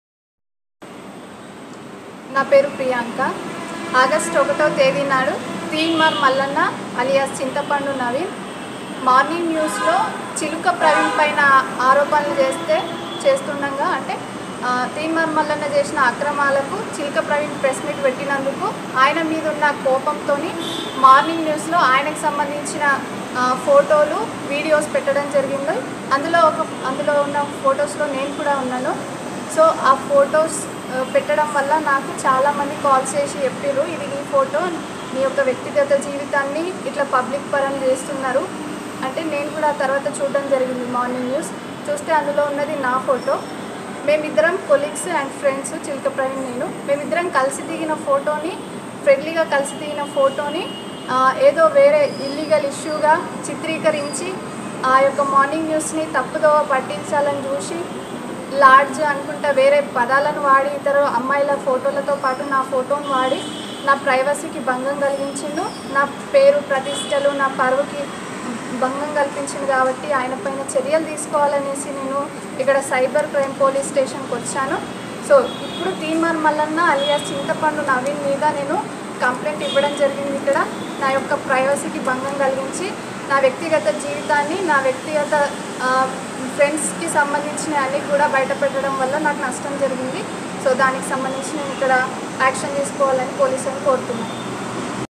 తీన్మార్ మల్లన్న మీద కేసుకుకారణం అయిన ఈయువతి ఇచ్చినఫిర్యాదులో ఏముంది?తీన్మార్ మల్లన్న అరెస్ట్ తప్పదా
తీన్మార్ మల్లన్న అరెస్ట్ కారణం
ఈ యువతి ఇచ్చిన ఫిర్యాదే..
ఇంతకీ ఆ ఫిర్యాదులో ఉన్నది ఏమిటీ??